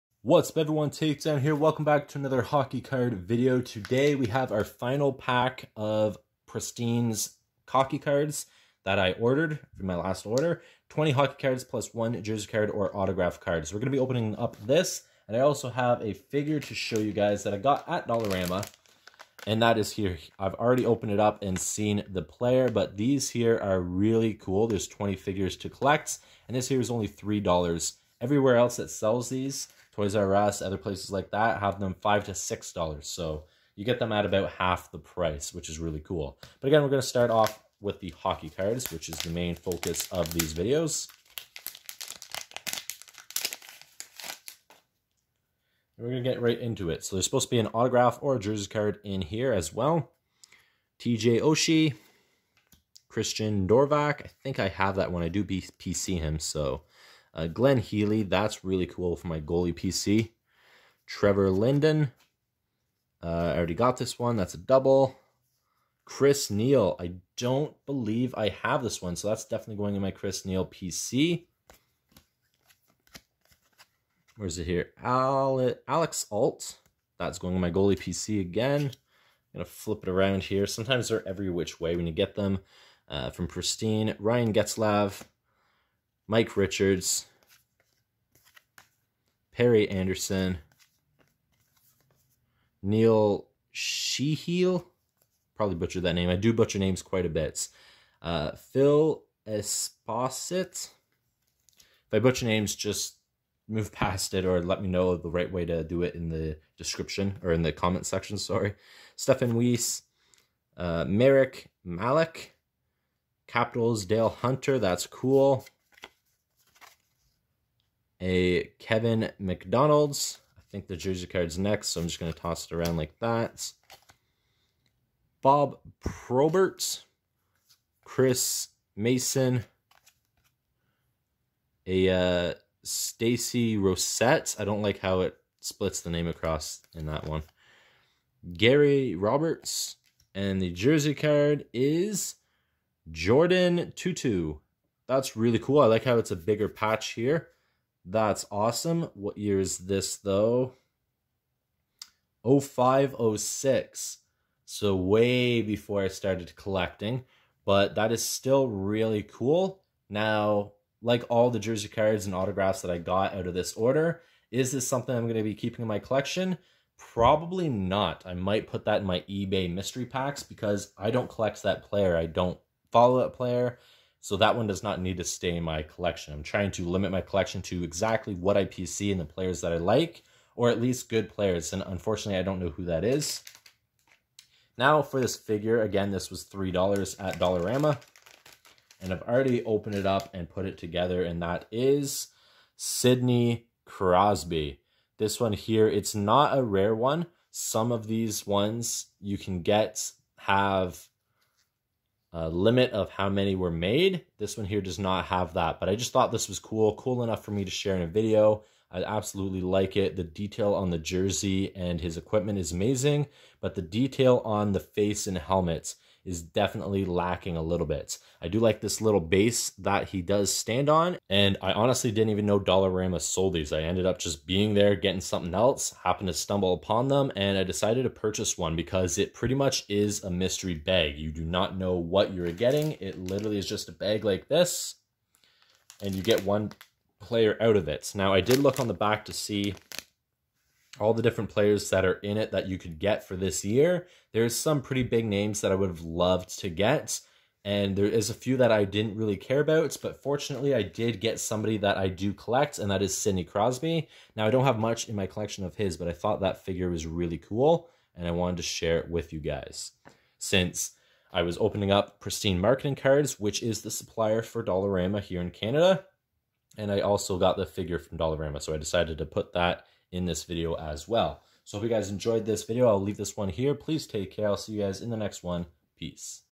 DOLLARAMA 20 CARD PACK + SIDNEY CROSBY MINI FIGURES
Today we open a 20 card pack from presstine marketing plus a mini figure